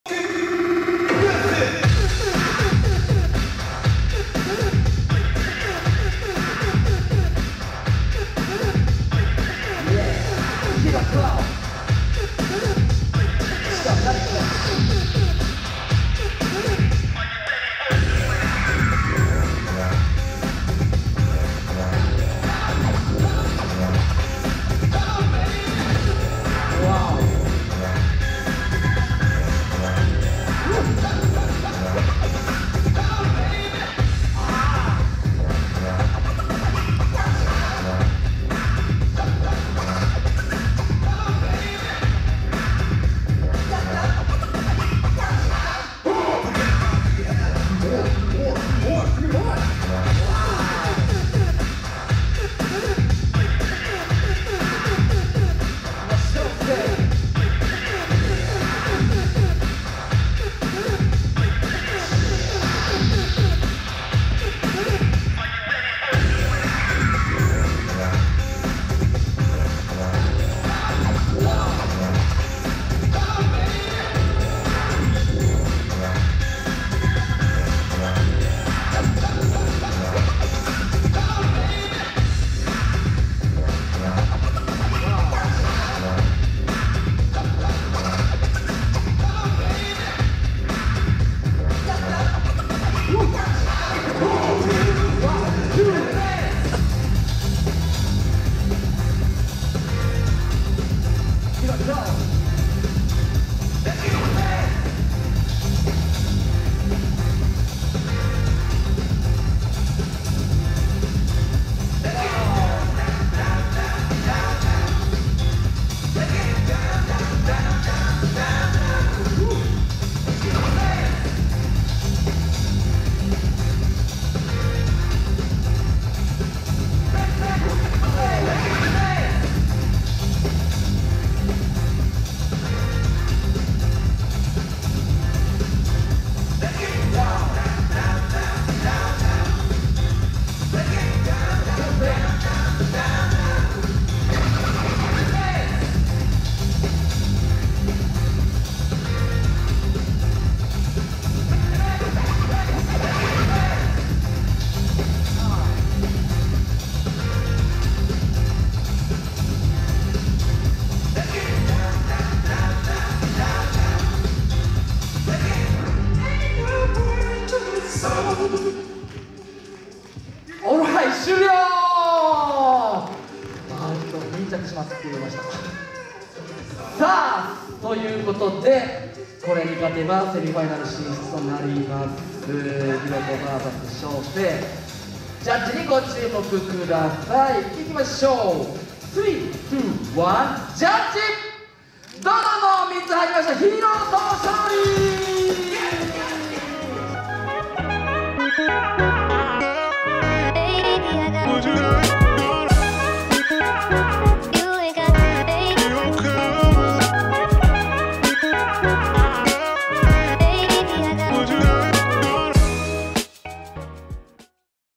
"SHOSEI vs HIRO10" SETTSU BREAKIN’ JAM オープン部門B-BOY 1vs1_TOP8
SETTSU BREAKIN’ JAM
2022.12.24 at 摂津市民文化ホール
オープン部門B-BOY1vs1

▼WINNER
HIRO10（石川/gun smoke breakers）
▼2nd PLACE
SHADE（群馬/ARIYA/臼井企画）
▼TOP4
HAYATO1（鹿児島/BDKMV/StruggleZ/HEROES）
WINGZERO（東京/FOUNDNATION）
▼TOP8
Taichi（福岡/九州男児）
RA1ON（兵庫/ReadytoRock/ AREA COMBINATION）
LIL'BOM（群馬/Bioneer Crew/もずくるー）
SHOSEI（石川/GOOD FOOT）

▼JUDGE
SHIE-CHAN, NARUMI, RYOMA, RYUJI